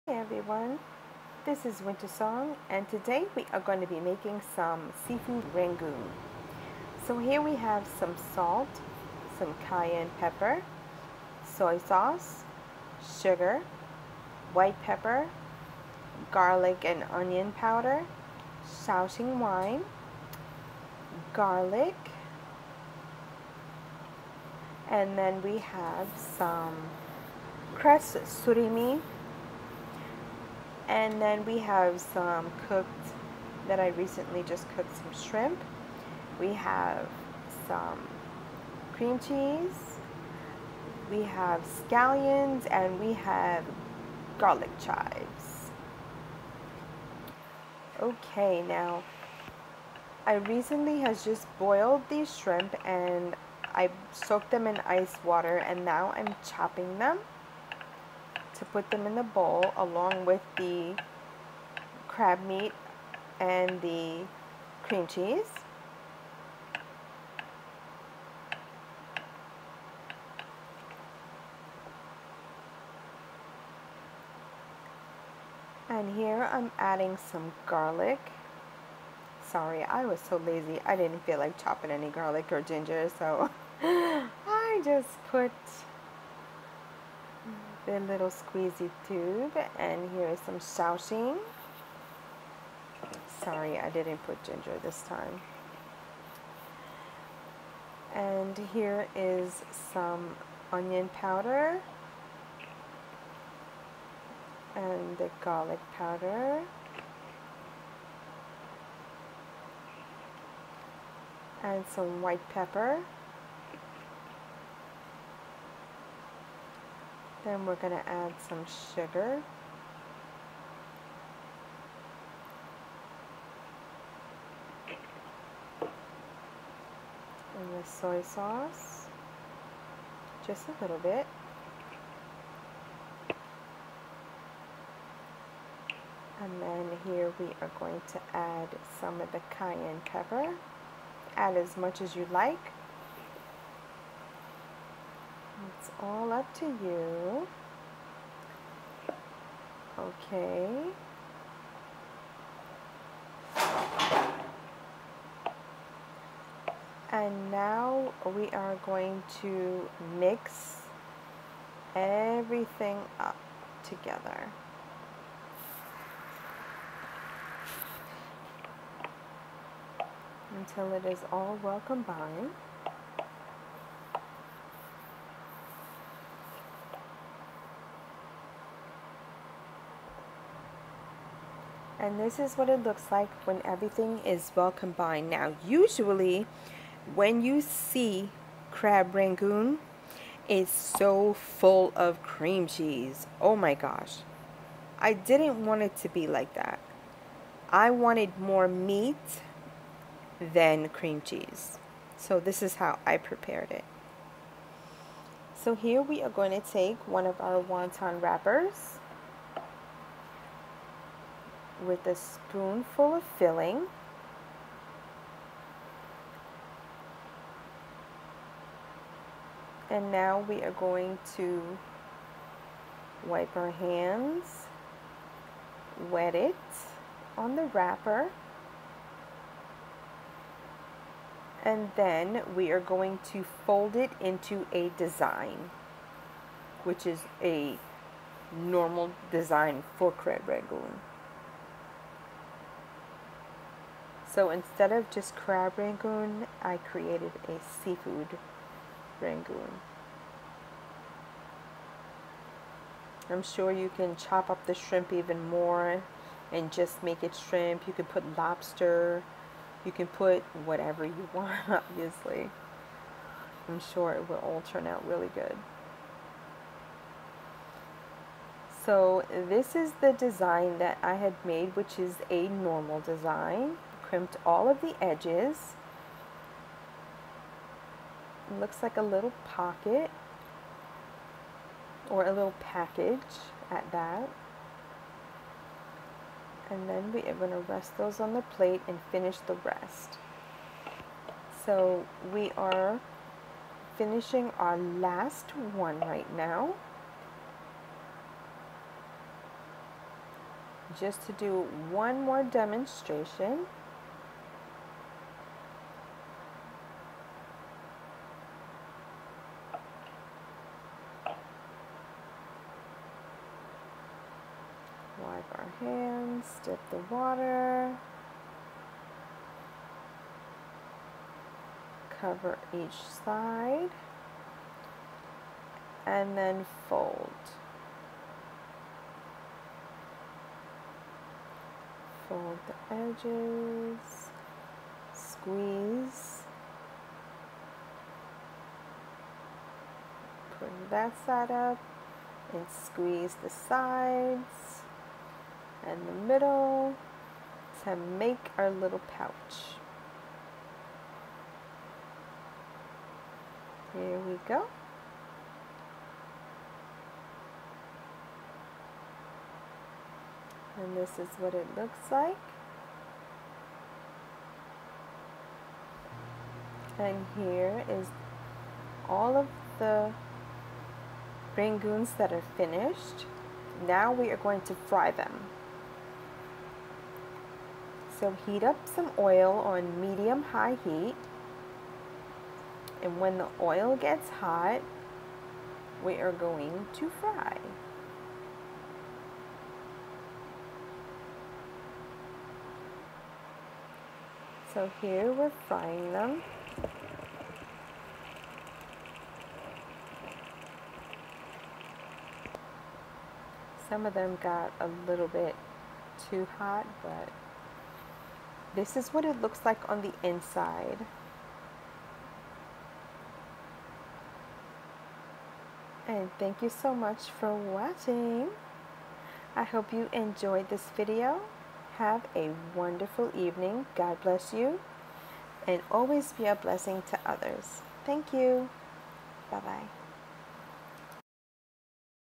Seafood Rangoon
Crab Rangoon is one of those snacks that you can't have just one of, but what about adding more inside to make it more interesting and not adding so much cream cheese...It came out absolutely amazing. I even had people fighting over them and hiding them so that no one else will find them!! LOL This recipe is very good. and the consistency is even better! Let's get going with this video!!